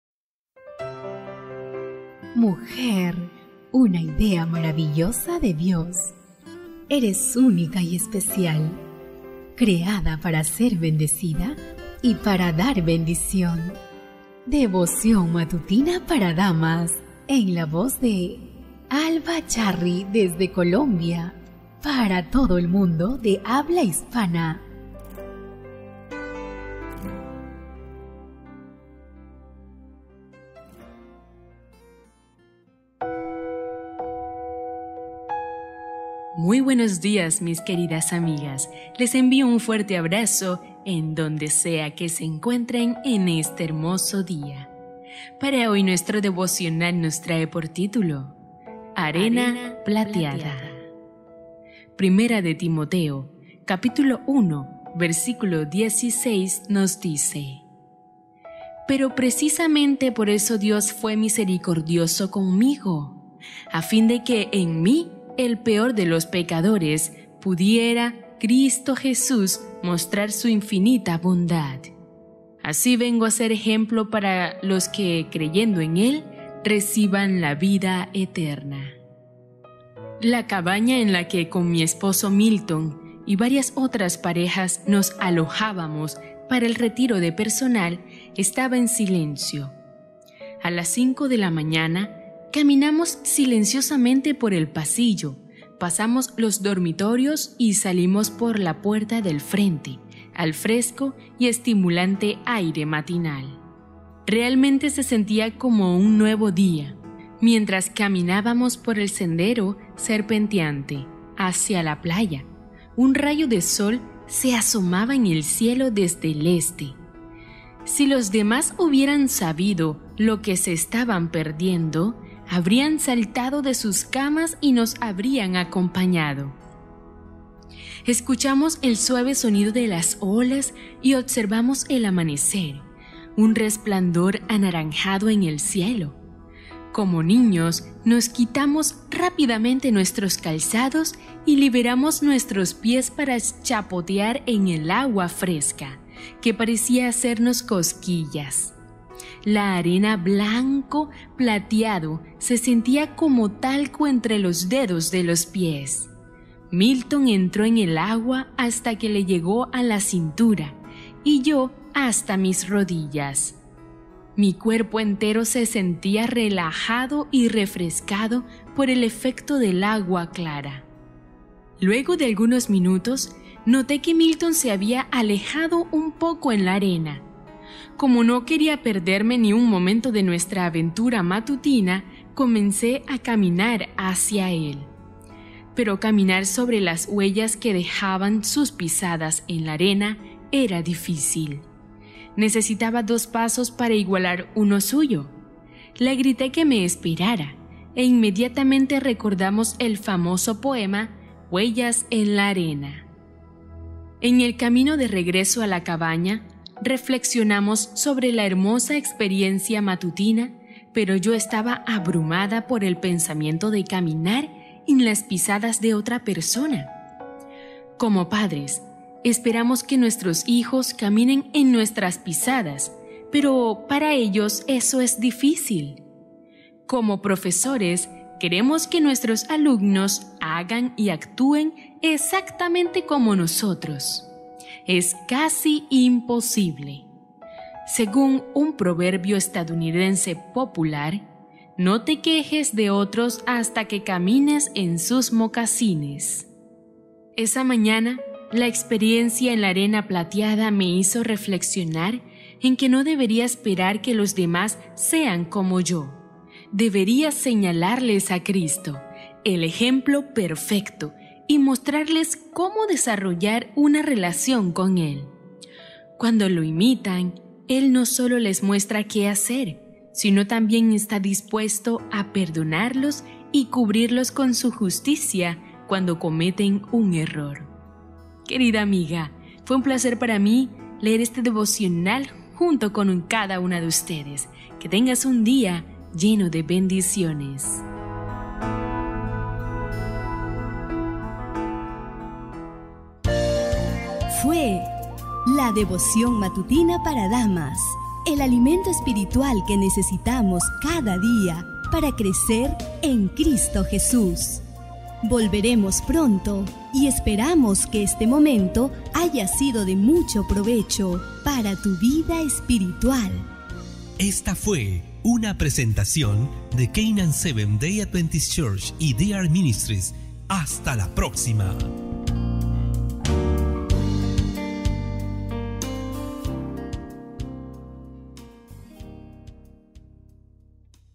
Devoción Matutina Para Damas - 04 de Septiembre del 2018
ARENA PLATEADA

"Pero precisamente por eso Dios fue misericordioso conmigo, a fin de que en mí el peor de los pecadores, pudiera Cristo Jesús mostrar su infinita bondad. Así vengo a ser ejemplo para los que creyendo en él, recibirán la vida eterna" (1 Tim. 1: 16).

La cabaña en la que con mi esposo Milton y varias otras parejas nos alojábamos para el retiro de personal, estaba en silencio. A las cinco de la mañana caminamos silenciosamente por el pasillo, pasamos los dormitorios y salimos por la puerta del frente, al fresco y estimulante aire matinal. Realmente se sentía como un nuevo día. Mientras caminábamos por el sendero serpenteante hacia la playa, un rayo de sol se asomaba en el cielo desde el este. Si los demás hubieran sabido lo que se estaban perdiendo, habrían saltado de sus camas y nos habrían acompañado. Escuchamos el suave sonido de las olas y observamos el amanecer, un resplandor anaranjado en el cielo. Como niños, nos quitamos rápidamente nuestros calzados y liberamos nuestros pies para chapotear en el agua fresca, que parecía hacernos cosquillas. La arena blanco plateado se sentía como talco entre los dedos de los pies. Milton entró en el agua hasta que le llegó a la cintura, y yo, hasta mis rodillas. Mi cuerpo entero se sentía relajado y refrescado por el efecto del agua clara.

Luego de algunos minutos, noté que Milton se había alejado un poco en la arena. Como no quería perderme ni un momento de nuestra aventura matutina, comencé a caminar hacia él. Pero caminar sobre las huellas que dejaban sus pisadas en la arena era difícil. Necesitaba dos pasos para igualar uno suyo. Le grité que me esperara, e inmediatamente recordamos el famoso poema "Huellas en la arena".

En el camino de regreso a la cabaña, reflexionamos sobre la hermosa experiencia matutina, pero yo estaba abrumada por el pensamiento de caminar en las pisadas de otra persona. Como padres, esperamos que nuestros hijos caminen en nuestras pisadas, pero para ellos eso es difícil. Como profesores, queremos que nuestros alumnos hagan y actúen exactamente como nosotros. Es casi imposible. Según un proverbio estadounidense popular: "No te quejes de otros hasta que camines en sus mocasines".

Esa mañana, la experiencia en la arena plateada me hizo reflexionar en que no debería esperar que los demás sean como yo. Debería señalarles a Cristo, el ejemplo perfecto, y mostrarles cómo desarrollar una relación con él. Cuando lo imitan, él no solo les muestra qué hacer, sino también está dispuesto a perdonarlos y cubrirlos con su justicia cuando cometen un error.

GLORIA GREGORY

Narrado por: Alba Charris
Lecturas Devocionales para Damas 2018
Bendecida
Por: Ardis Stenbakken